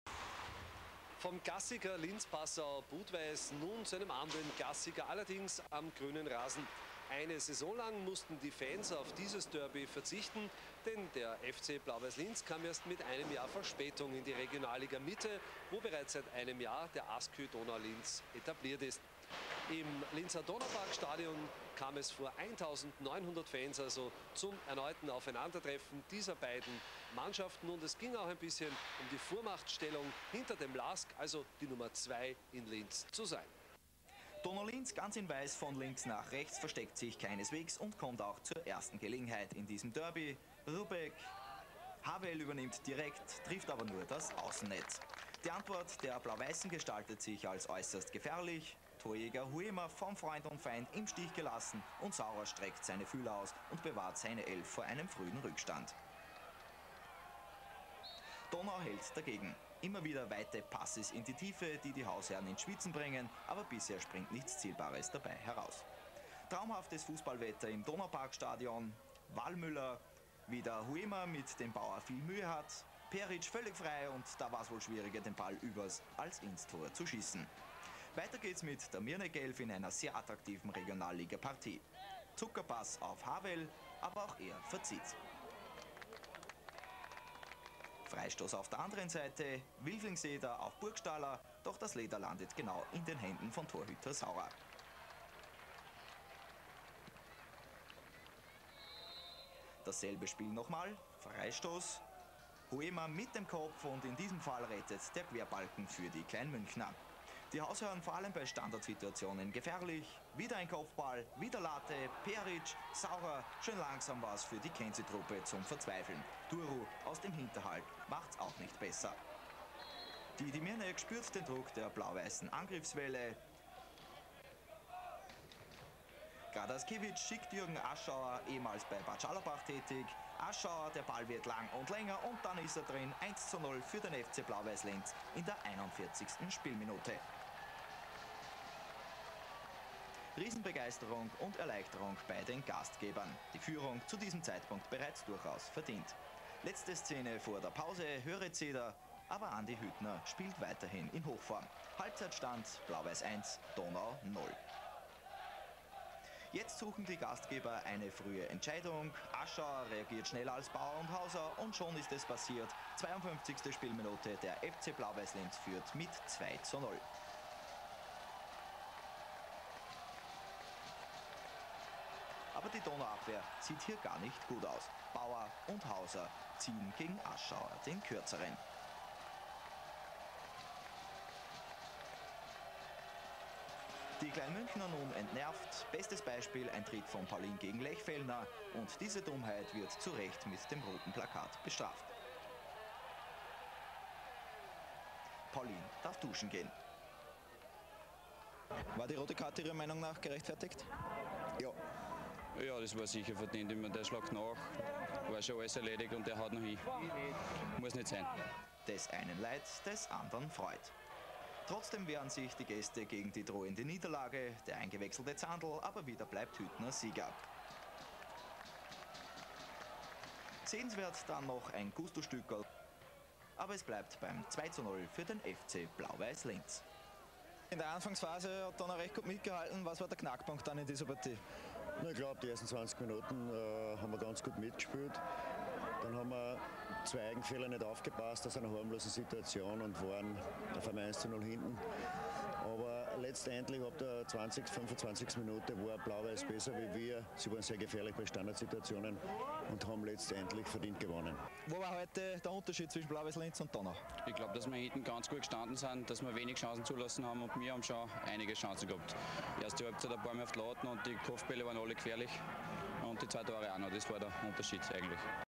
FC Blau-Weiss Linz - ASKÖ Donau Linz 2:0, 1. 9. 2000
Spiel zur 6. Runde der Regionalliga Mitte in der Saison 2000/01 am 1. September 2000 zwischen FC Blau-Weiss Linz und SK Eintracht Wels, Endstand 2:0 vor 1.800 Zuschauern im Donaupark-Stadion Linz